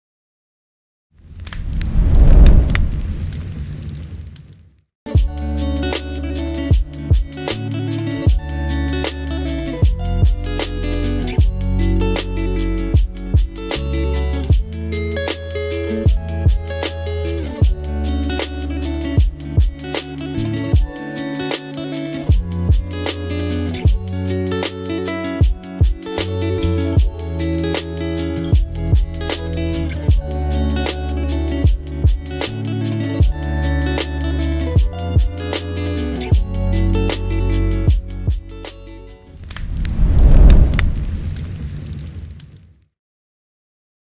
Real Flame - 58" Riverside Large Oval LP Fire Bowl
Transform your outdoor space into a relaxing and inviting retreat with the natural finish and harmonious form of the Riverside Large Oval Propane Fire Bowl. Crafted from durable MGO, this substantial outdoor gas fire pit has a reliable electronic push button ignition and includes a stainless steel burner, gray pumice stone and a protective storage cover. Complete the look with a matching Real Flame® tank cover that conceals your propane tank and doubles as a side table.

Riverside592LP 1080p GeneralUse WithSound